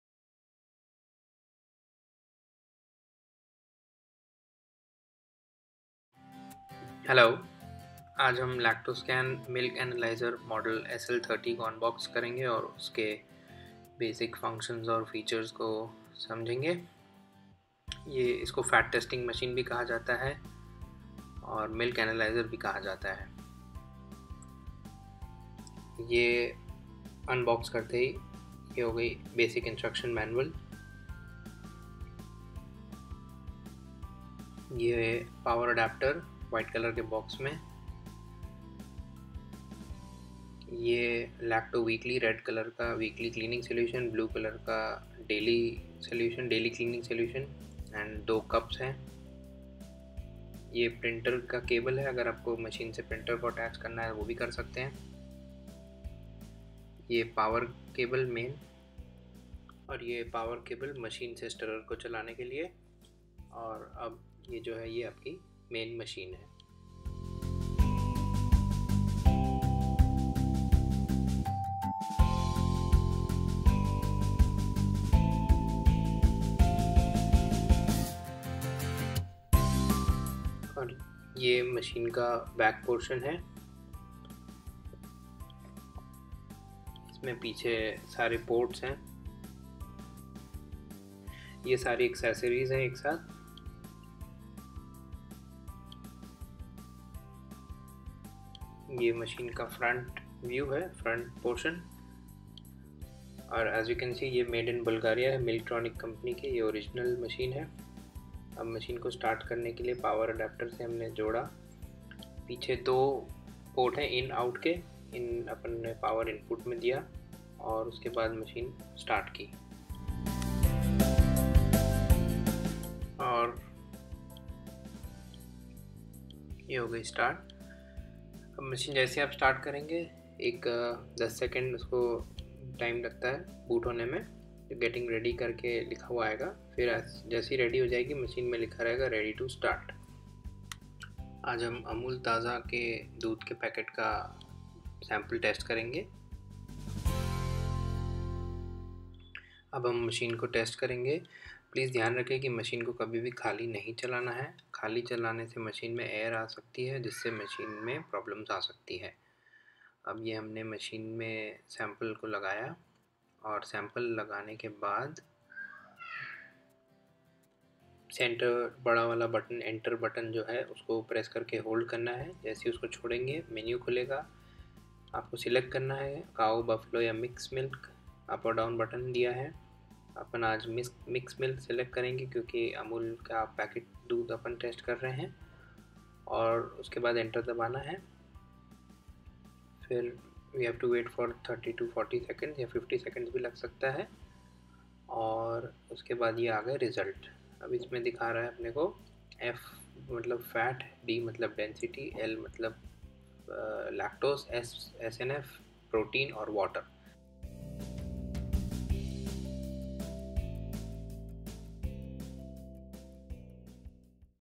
Milk Analyser Fat testing machine (हिंदी डेमो)
This machine is used by all Dairy Farms, sweet Shops and other milk related businesses for determine the quality of milk.
This machines gives the following values of milk
Fat, Density, Lactose, SNF, Protein and Water.

Regards
Kamla Enterprises
Raipur